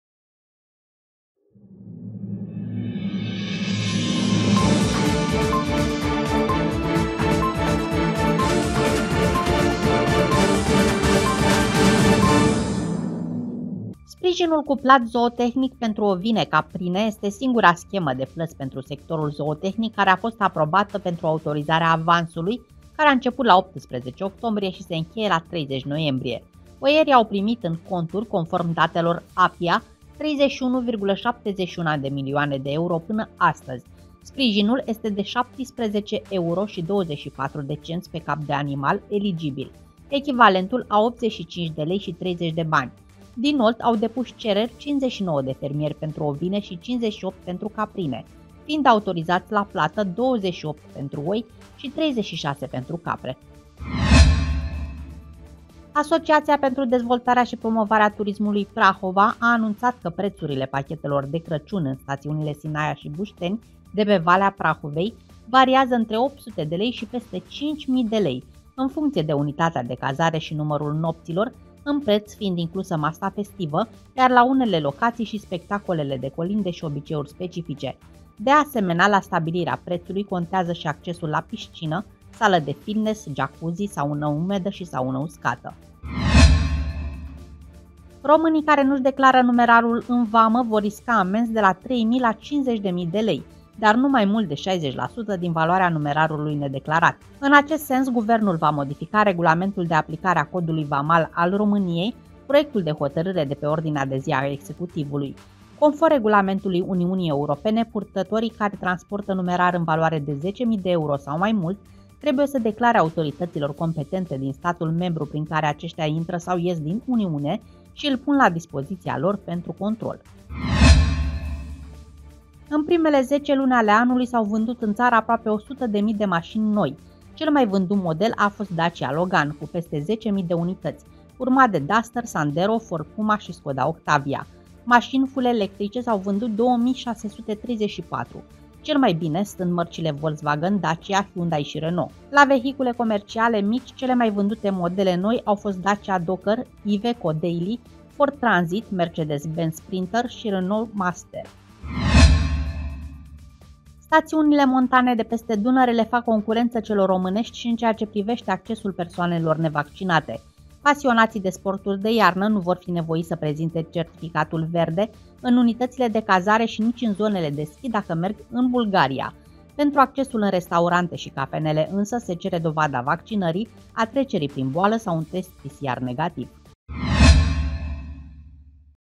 Realitatea pe scurt 08.11.2021
Crescătorii de ovine și caprine au primit banii
Crăciun la stațiune și cu peste 5000 lei
Amendă pentru banii nedeclarați la vamă
Ce mașini noi cumpără românii
Ski în Bulgaria, fără certificat covid pentru români